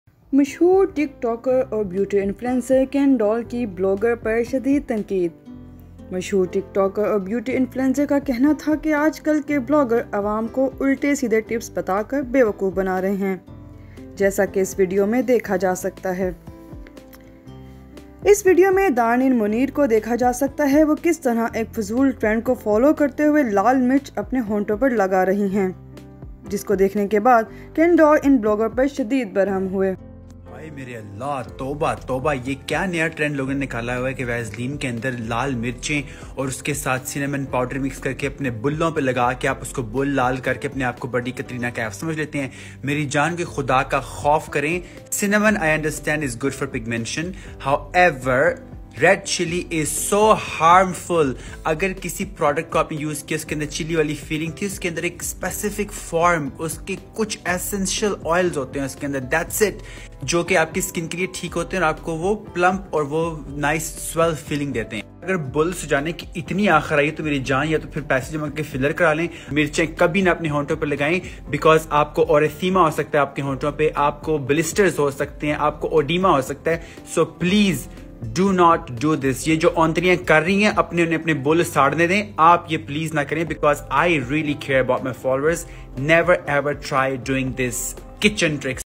Ken doll aka Adnan Zafar bashes on dananeer mubeen | life707
Ken doll aka adnan zafar first international  Pakistani beauty influencer is bashes dananeer mobeen, urf Pawri Girl for applying chili on the lips